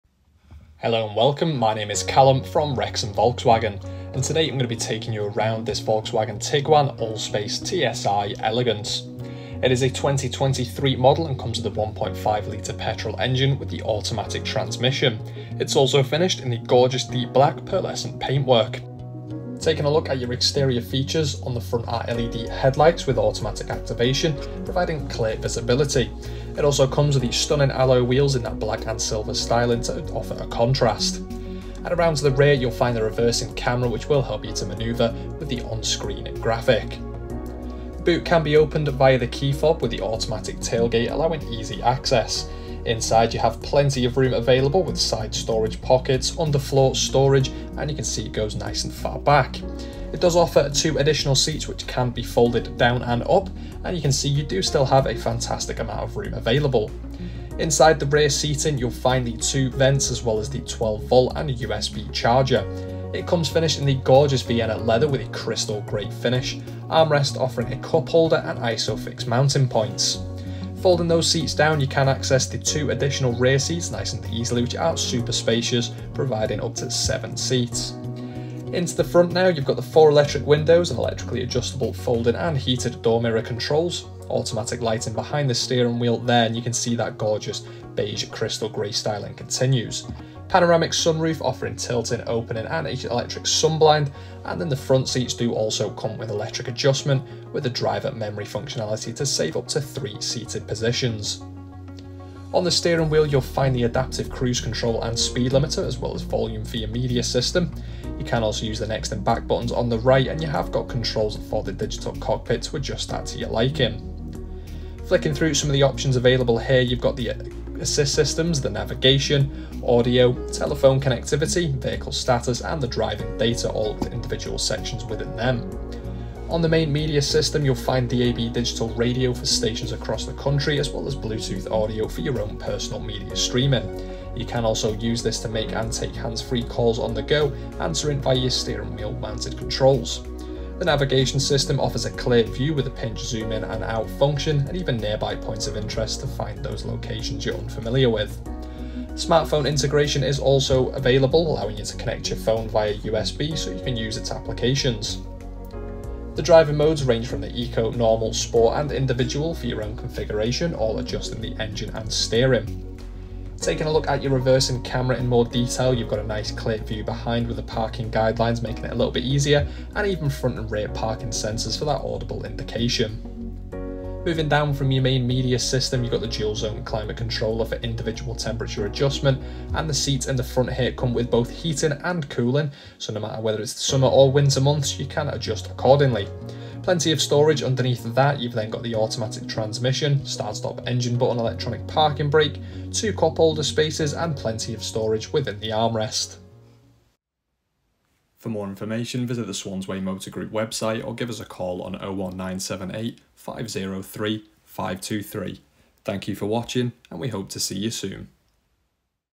Approved Used Volkswagen Tiguan Allspace TSI Elegance | Wrexham Volkswagen | DF23OXT
Just arrived in stock this fantastic approved used  Volkswagen Tiguan Allspace TSI Elegance in black, now available for sale from Wrexham Volkswagen.

Reg Number: DF23OXT
Fuel: Petrol
Transmission: Automatic
Mileage: 6,244

Top Features
- Satellite Navigation
- Heated & Cooled Seats
- Reversing Camera